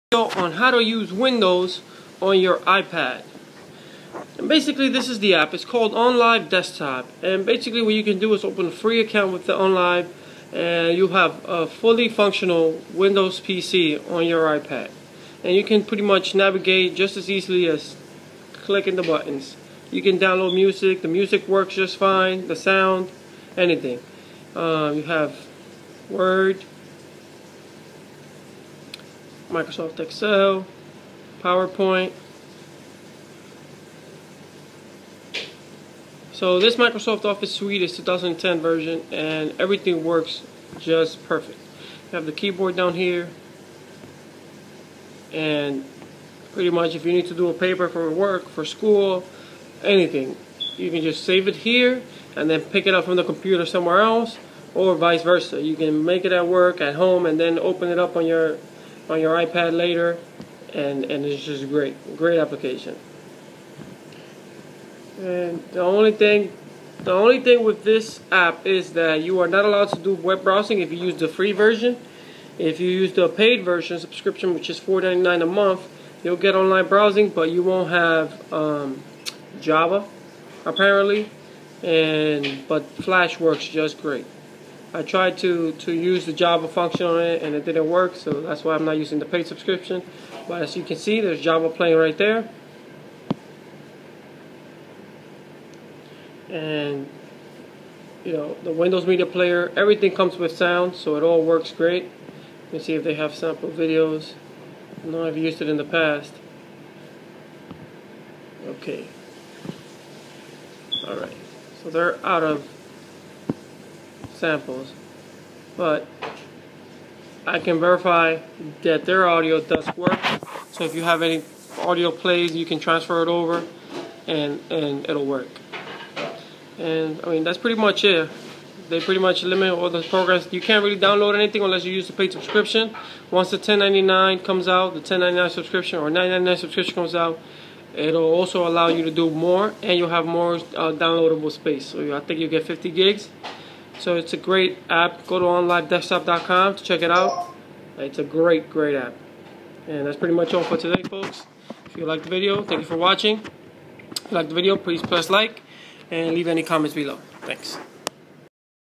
How to get windows for your iPad "onlive desktop" no jailbreak needed
Fully functional Windows operating system on your iPad.

Download and install Onlive Desktop and you will have a fully functional windows operating system on your iPad